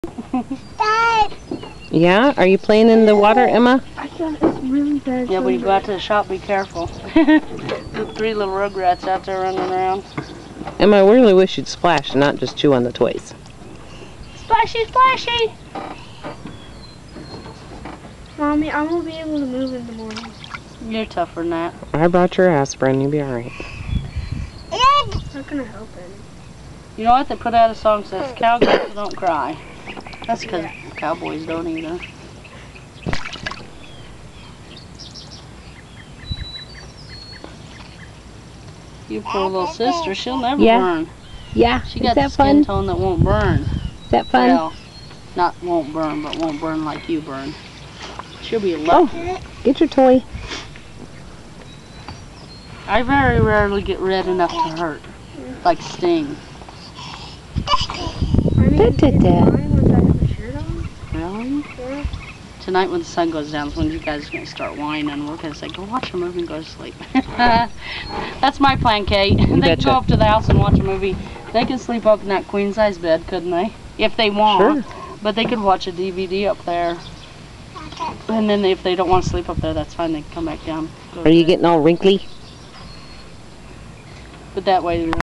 Emma playing in the water
Emma playing in the baby pool - she's doing more toy chewing than anything, but she calls for the dog at one point which is cute :)